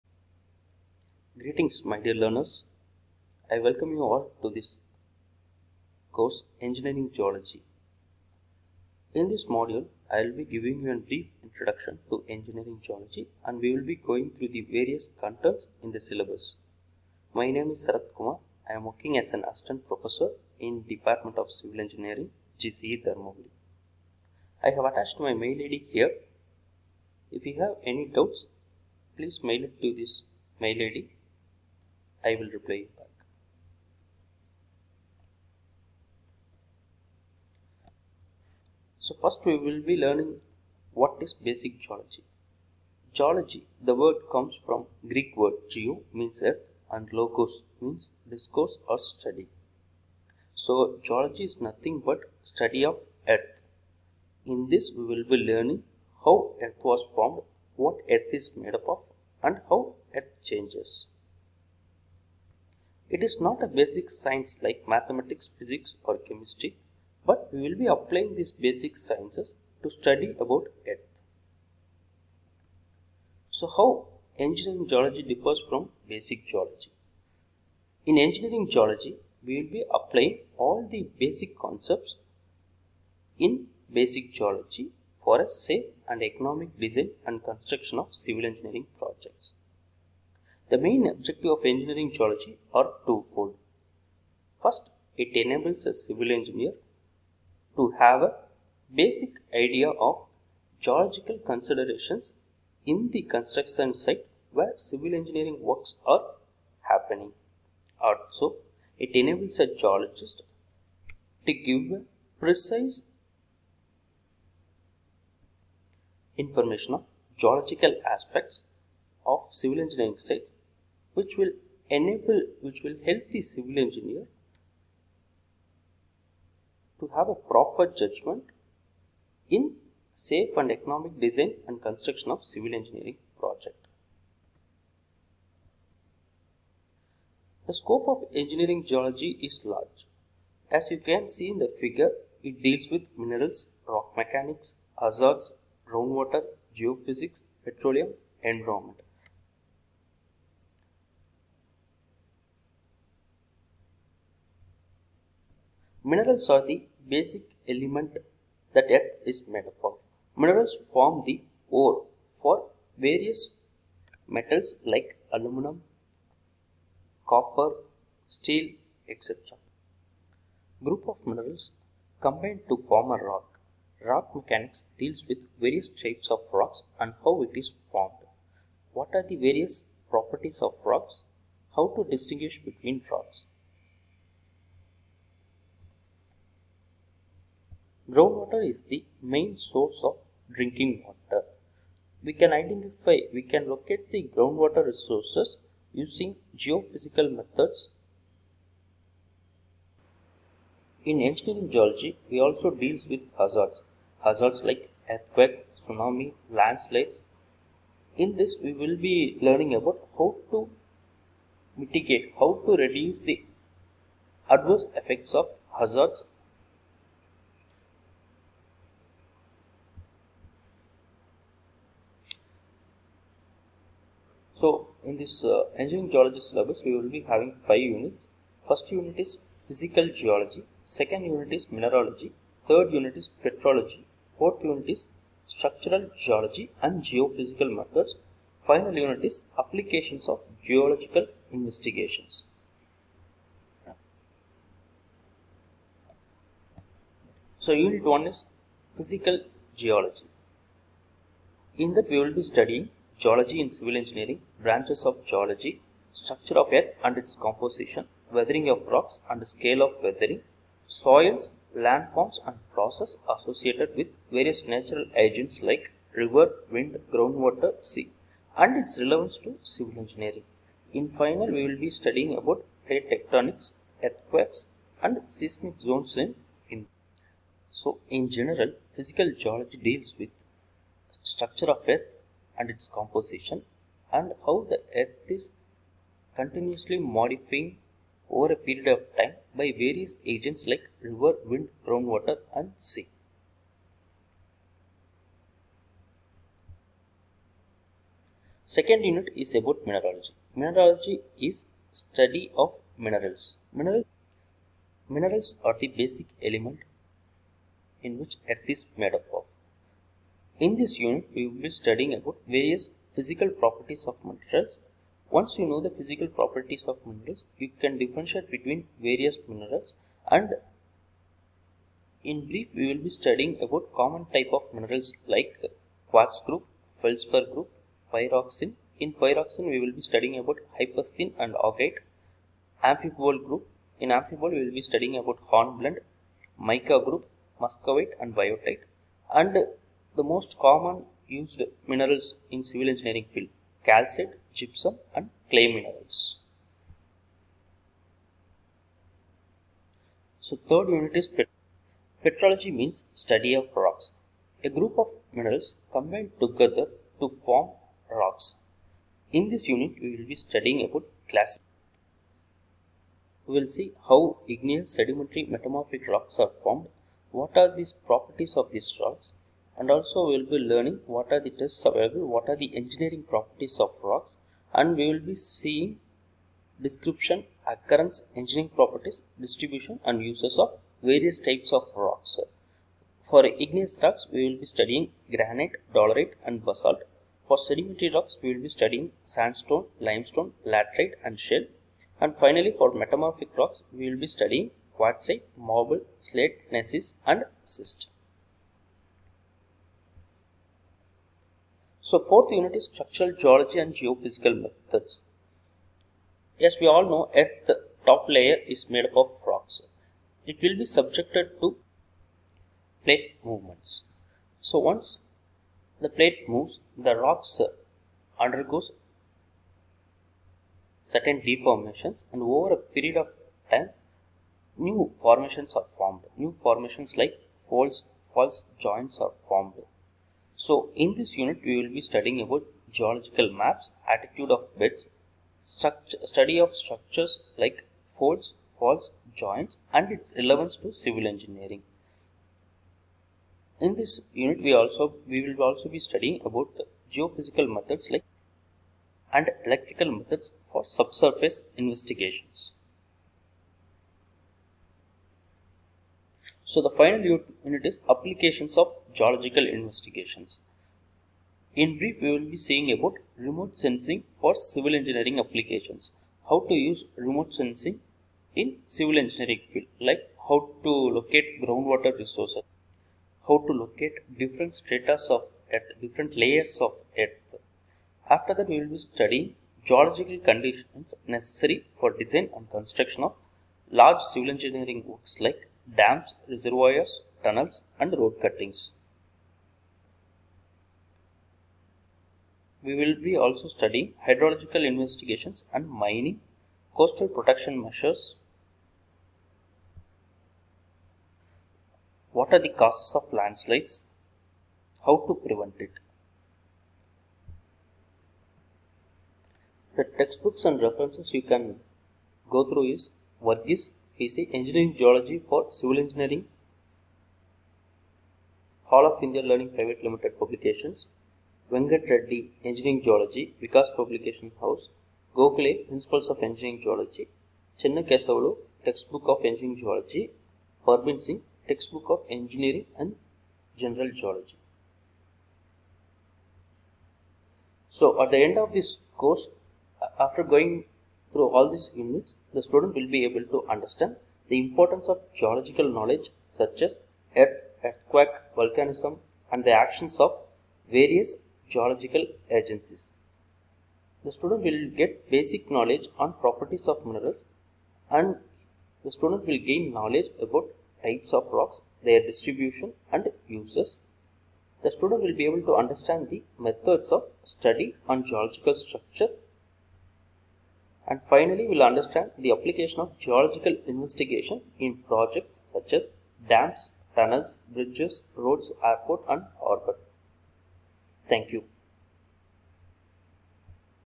Introduction to ENGINEERING GEOLOGY and CE8392 SYLLABUS
CE8392 ENGINEERING GEOLOGY
BE CIVIL ENGG III SEM
BREIF INTRODUCTION TO THE COURSE